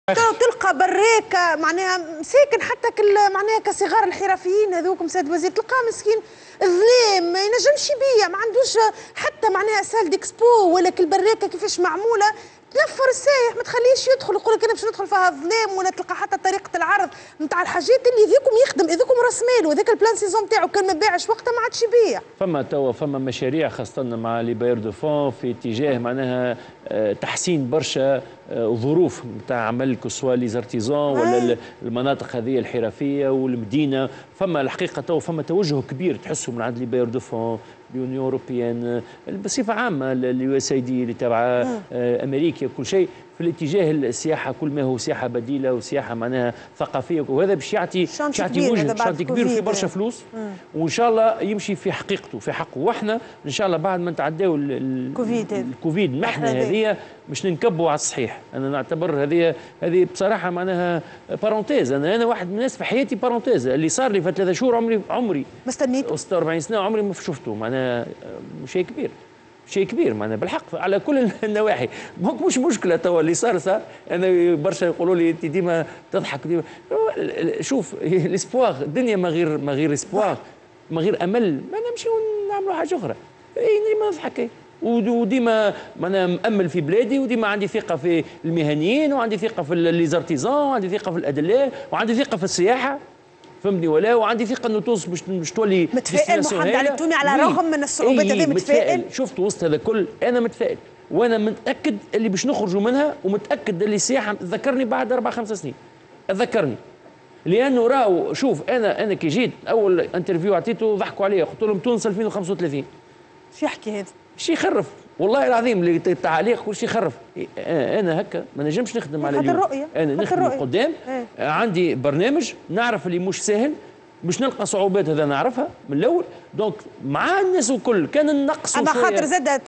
محمد علي التومي اليحدث في تونس: عمري 46سنة عمري متصورت الي صار في ث
محمد علي التومي وزير السياحة و الصناعات التقليدية ليحدث في تونس: عمري 46سنة عمري متصورت الي صار في ثلاث اشهر هاذم...